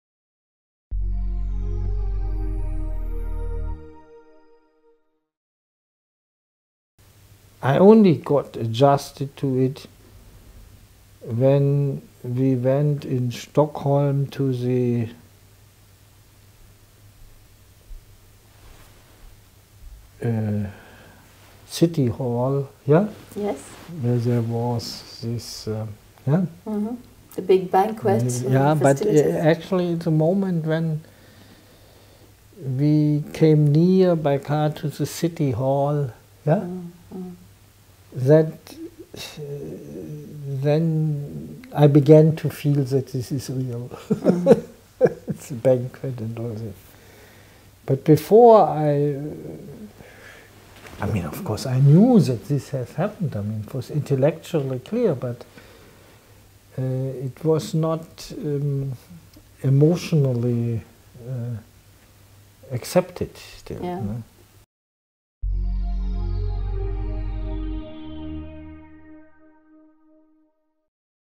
"I began to feel that this is real." Reinhard Selten on his Nobel Week experience
Interview with Professor Reinhard Selten at the 1st Meeting of Laureates in Economic Sciences in Lindau, Germany, September 1-4, 2004. Interviewer is freelance journalist Marika Griehsel.